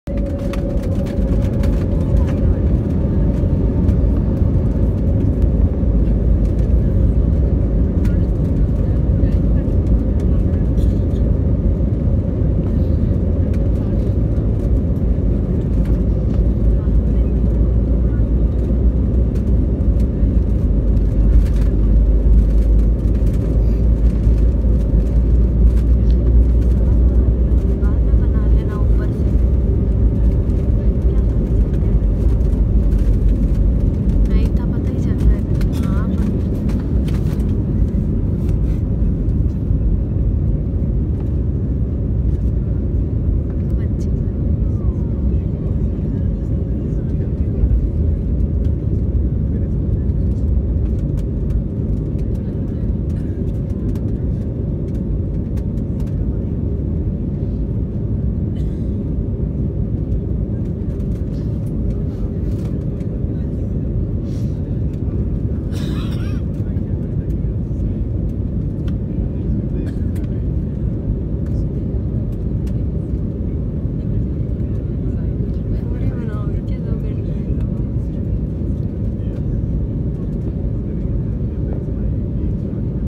Plane Take off || Jaipur Airport || Indigo Flight || Runway
Jaipur airport flight to kerla.